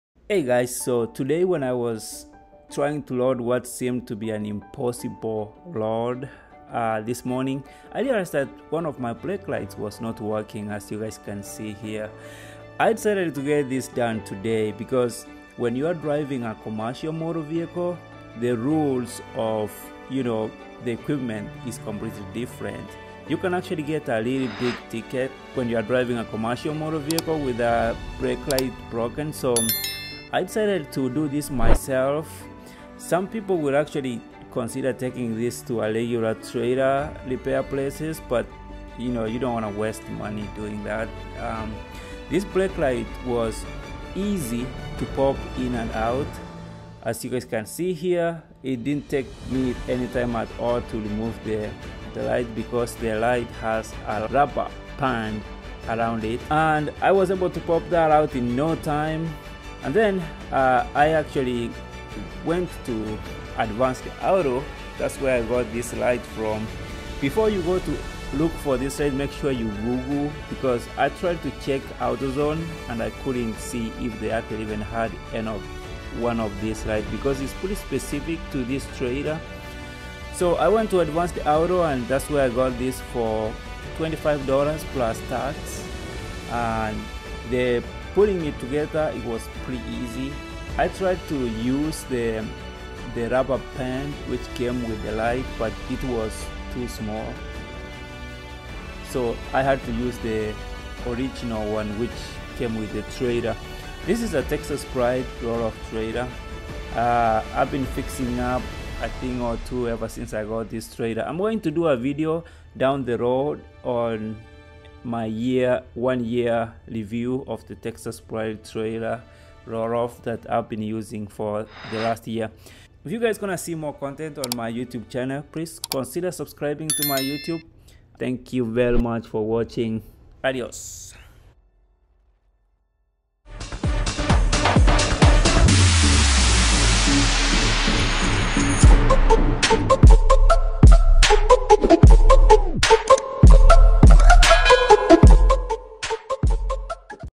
AVOID BEING PULLED OVER BY MAINTAINING YOUR EQUIPMENT. WAYS TO FIX YOUR TRAILER LIGHT.
make sure to mention your trailer for commercial or recreation purposes .